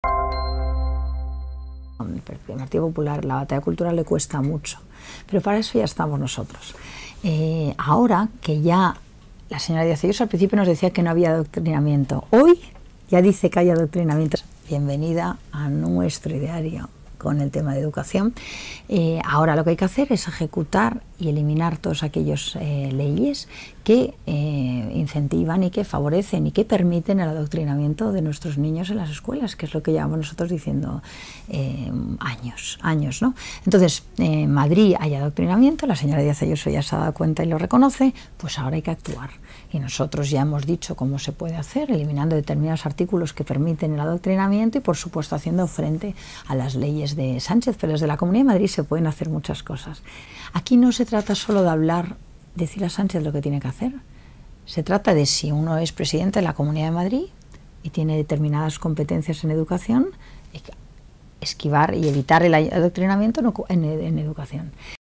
Monasterio celebra que Ayuso reconozca que hay adoctrinamiento en las aulas
Madrid, 7 jun (EFE)(Imagen: Manuel Única).- La líder de Vox en la Comunidad de Madrid, Rocío Monasterio, ha celebrado que la presidenta regional, Isabel Díaz Ayuso, reconozca que hay "adoctrinamiento" en las aulas al recurrir este lunes el currículo de Bachillerato del Gobierno ante el Tribunal Supremo porque tiene "gran carga ideológica".